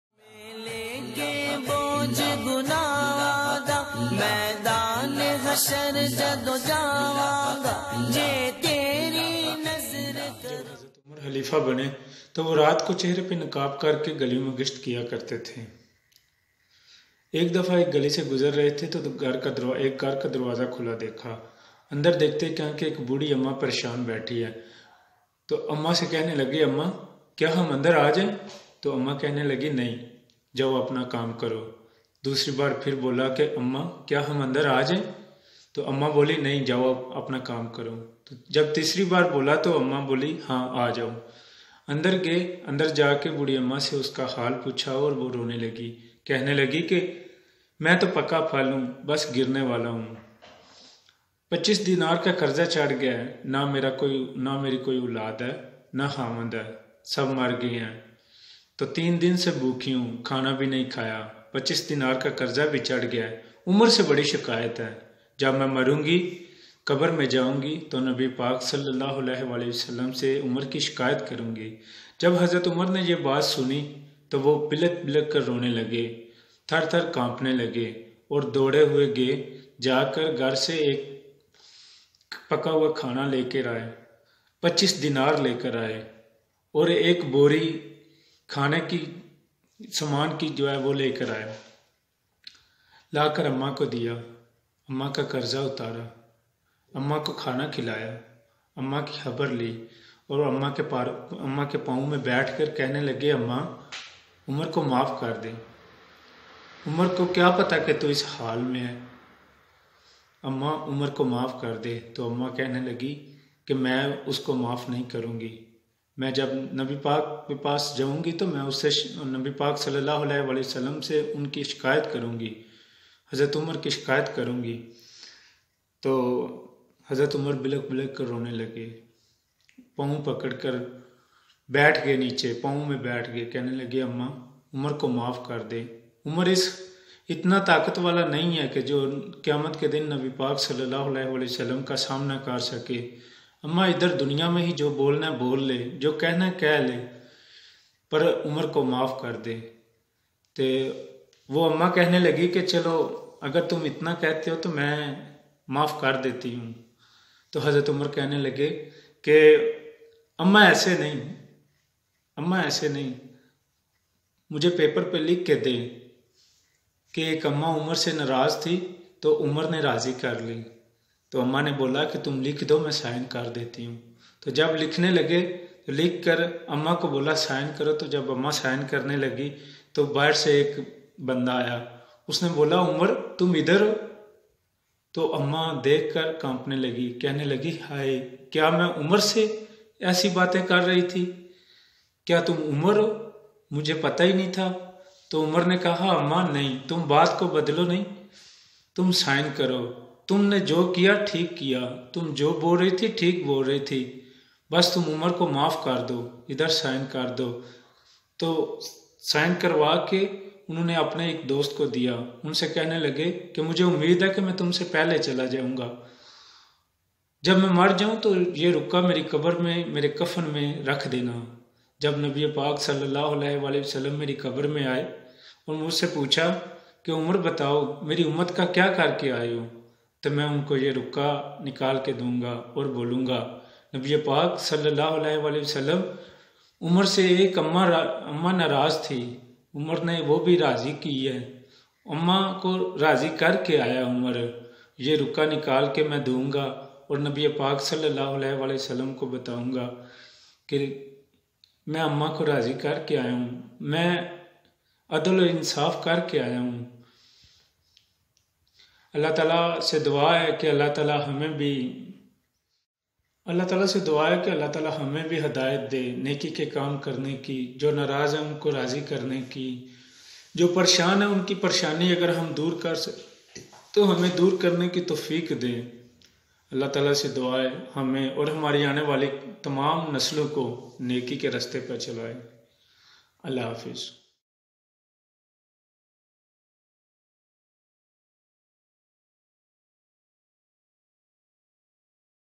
hazrat umar aur buri amma
ansari sahib itali,,hazrat umar aur buri amma ka kissa ,,,,aur unki madad karna ,,aur mafi mangna